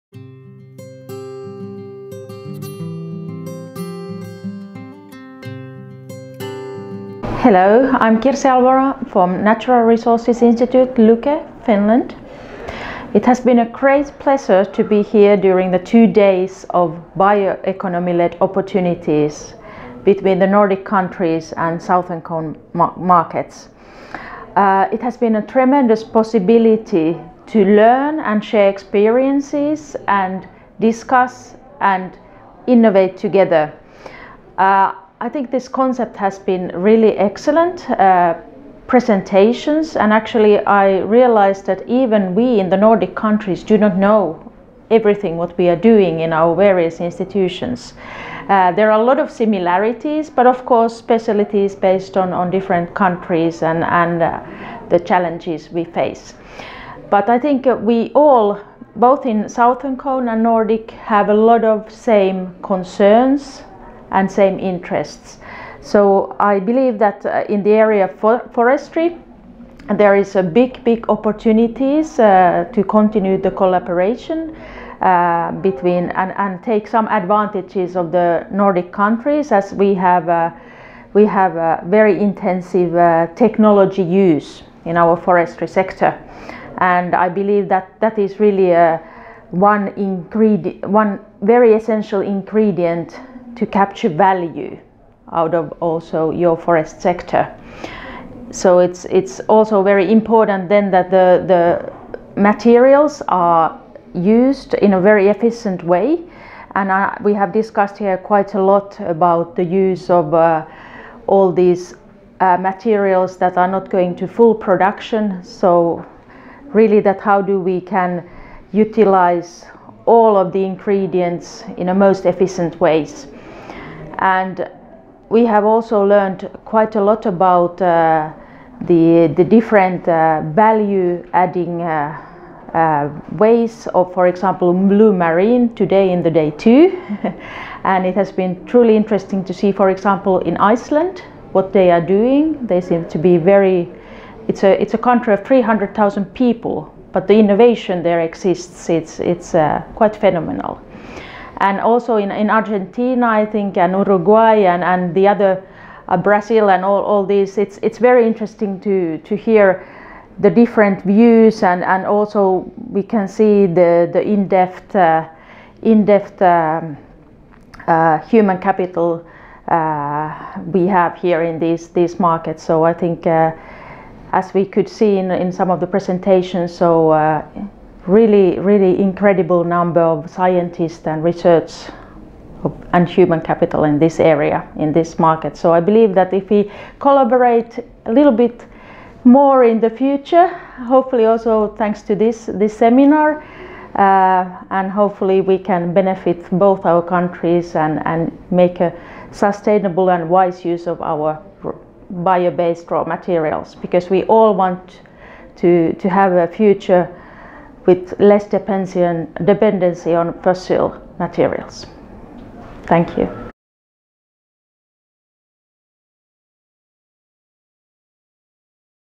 Minientrevistas CIECTI: Kirsi Arvelo
En el marco del último Seminario Internacional CIECTI 2018: “Bioeconomía y desarrollo: oportunidades a través de la cooperación entre los países nórdicos y el Cono Sur”, realizado el 18 y 19 de octubre de 2018 en Buenos Aires, hemos dialogado con referentes internacionales de la bioeconomía. Aquí compartimos las palabras de Kirsi Arvelo (LUKE, Finlandia).

Kirsi Arvelo es vicepresidenta de Natural Resources Institute Finland (LUKE), Finlandia.